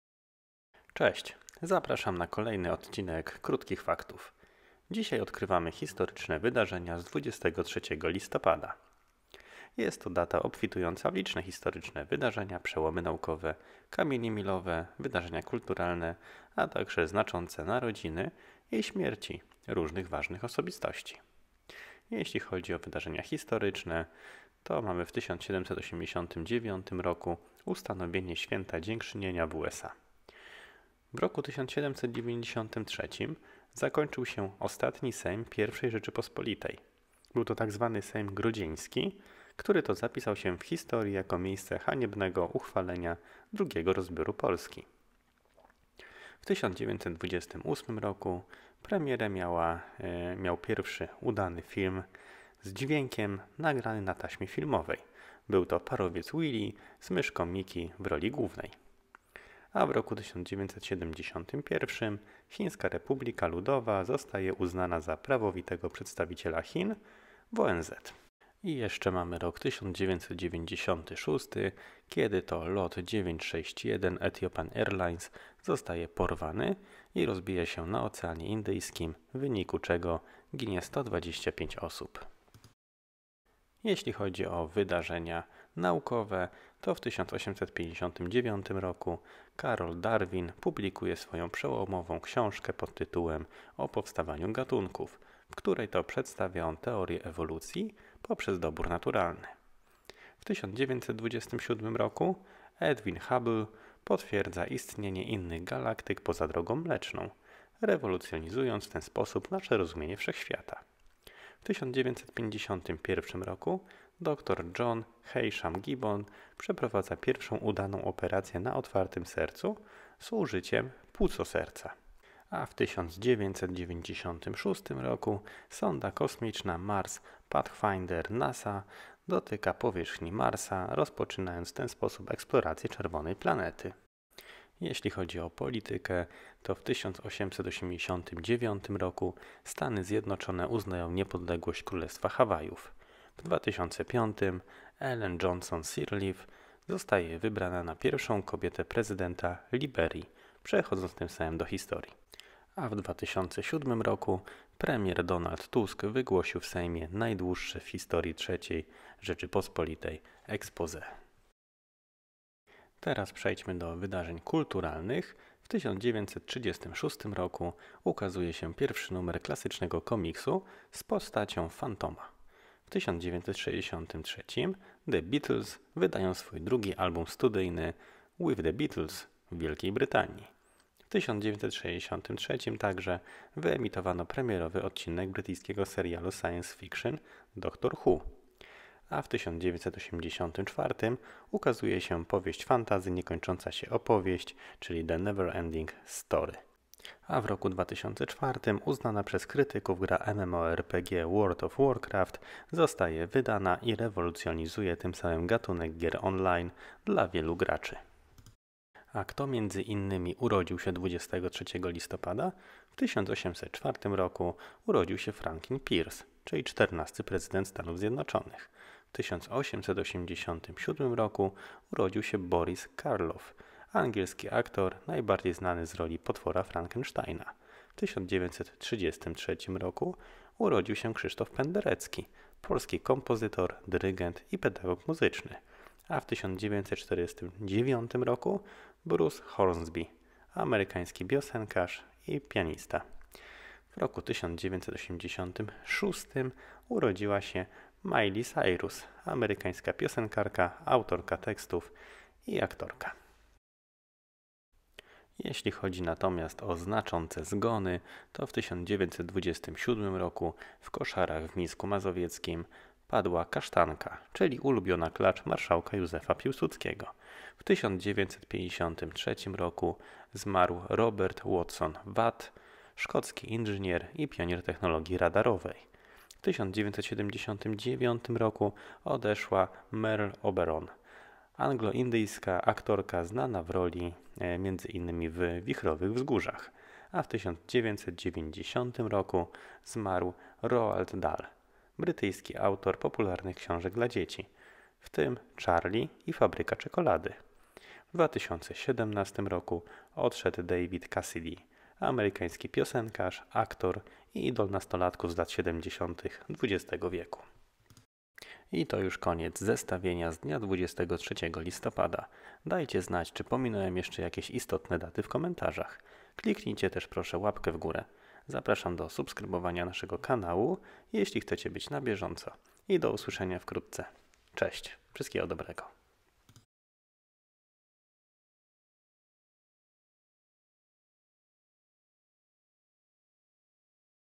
Co ciekawego wydarzyło się 23 LISTOPADA w przeszłości i historii? Krótkie Fakty
Co ciekawego w historii wydarzyło się w dniu 23 listopada? Jakie ważne wydarzenia historyczne, odkrycia naukowe, kulturalne i polityczne przełomy miały miejsce tego dnia? Kto znany i ważny urodził się tego dnia w przeszłości? A kto odszedł tego dnia z tego świata? Zapraszam na kolejny odcinek progrmau Krótkie Fakty gdzie analizujemy dzisiejszą datę 23 listopada pod kątem wydarzeń z przeszłości.